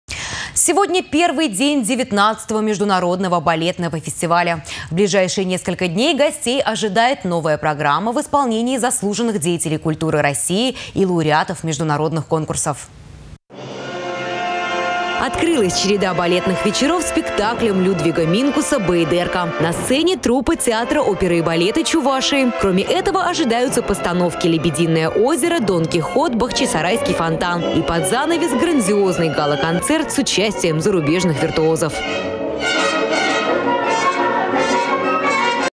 Открытие балетного фестиваля
Сегодня первый день 19 международного балетного фестиваля. В ближайшие несколько дней гостей ожидает новая программа в исполнении заслуженных деятелей культуры России и лауреатов международных конкурсов